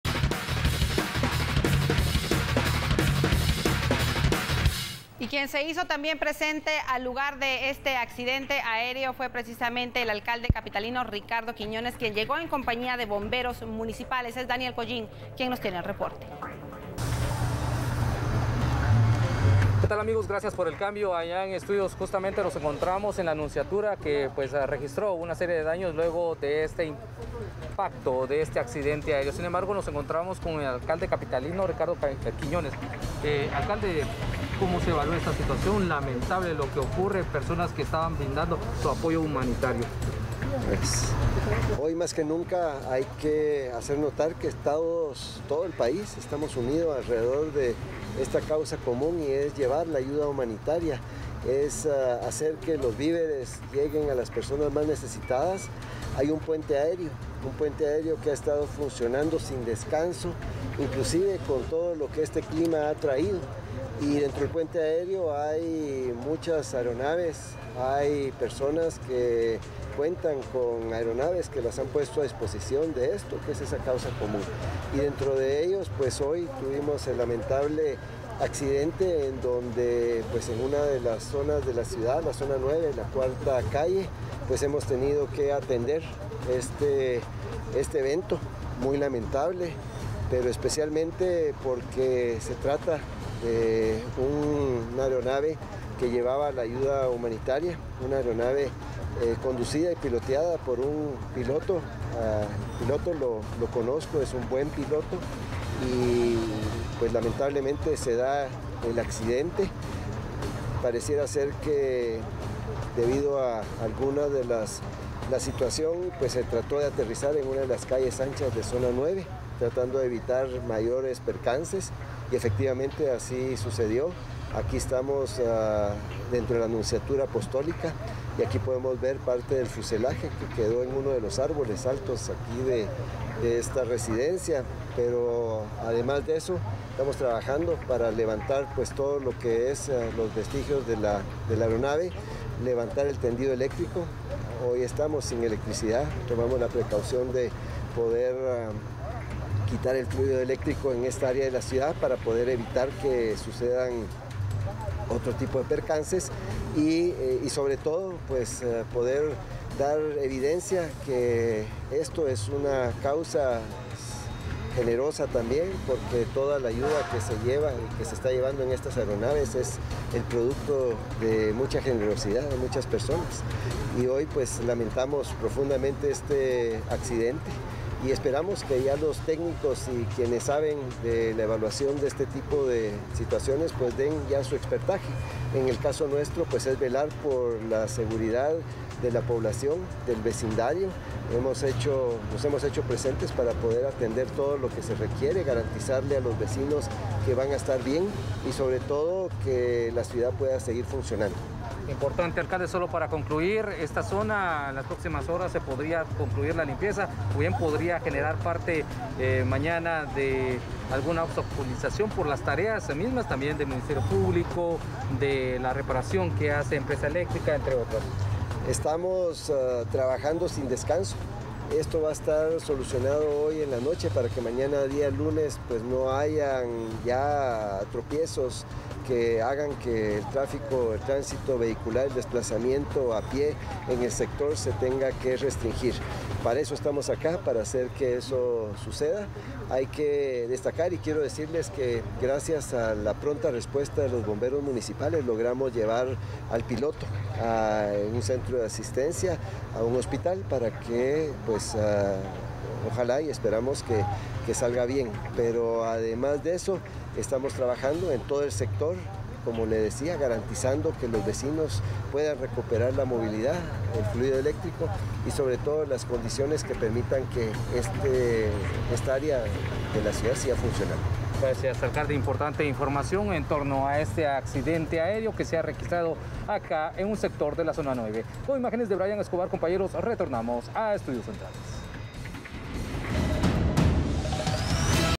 Alcalde capitalino llegó al lugar de accidente aéreo en zona 9 capitalina
Accidente aéreo re registró este domingo 08 de noviembre en un sector de zona 9 capitalina.